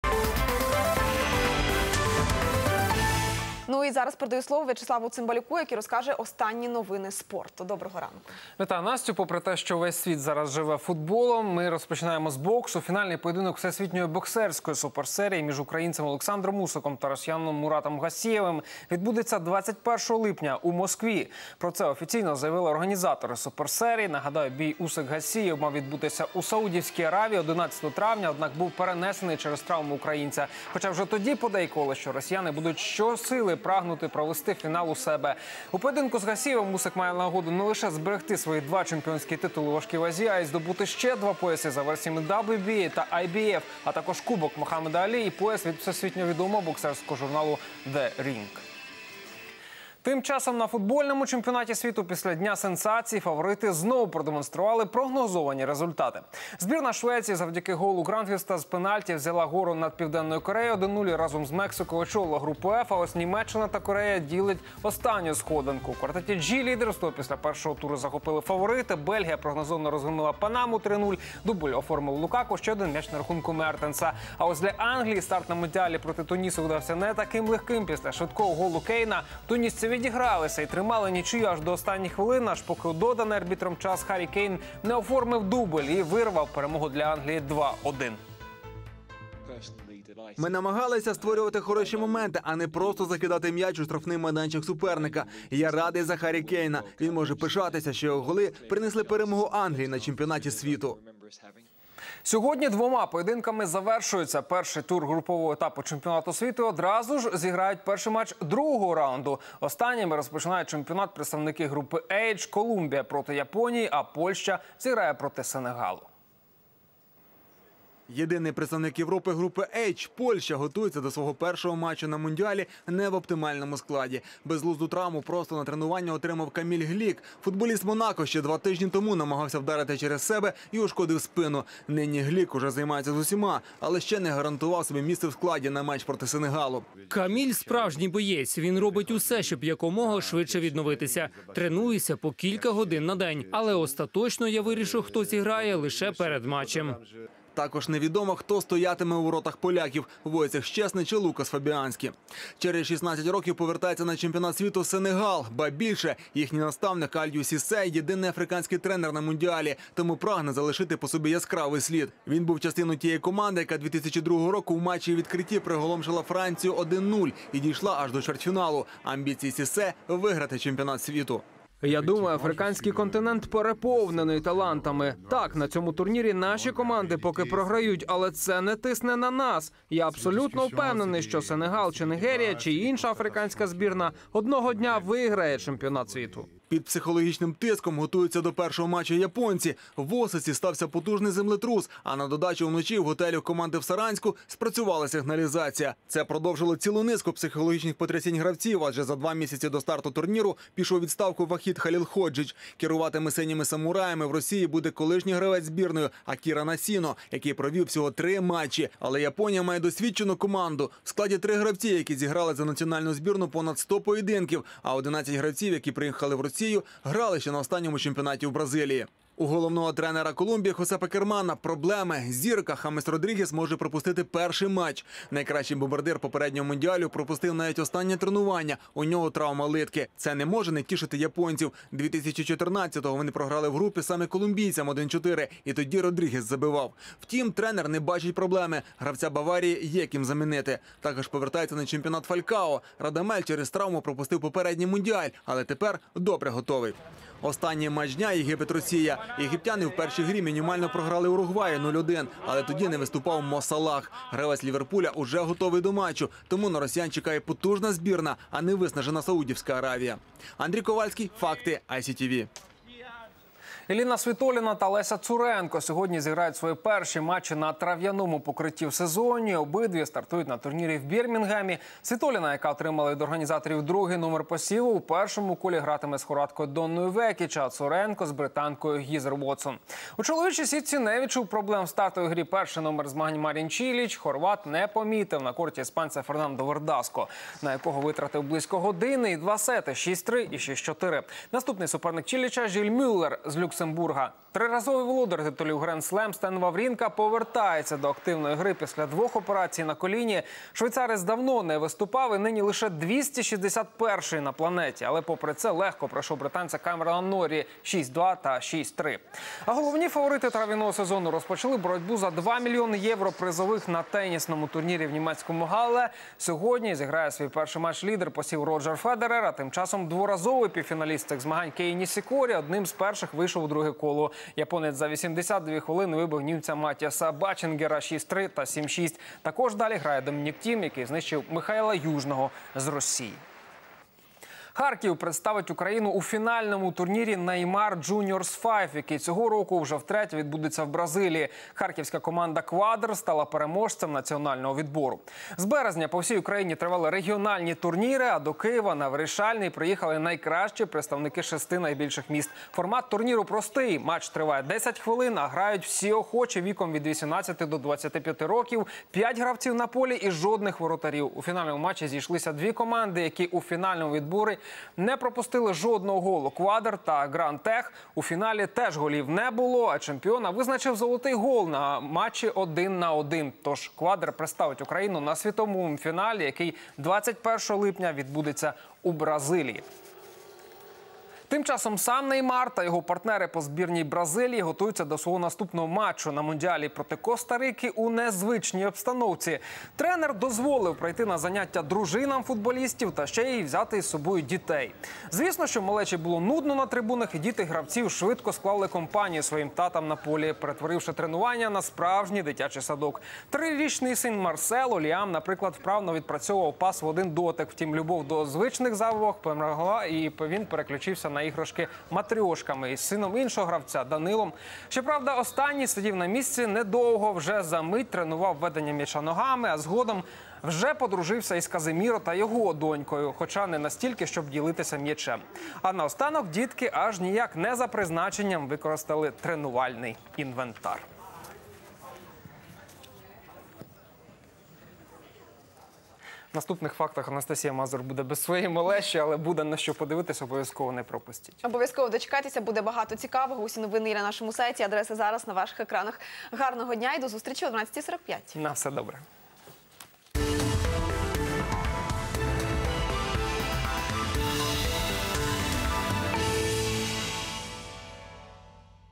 Факты. Спорт. 19 июня
Самые актуальные спортивные события 19 июня:

- Финальный поединок Всемирной боксерской суперсерии между украинцем Александром Усиком и русским Муратом Гассиевым, состоится 21 июля в Москве.
- На футбольном чемпионате мира после дня сенсаций, лидеры вновь продемонстрировали прогнозируемые результаты. Сборная Швеции, благодаря голу Гранквиста с пенальти взяла верх над Южной Кореей.
- Харьков представит Украину в финальном турнире Неймар Джуниорс Файв, который в этом году уже в третий раз пройдет в Бразилии.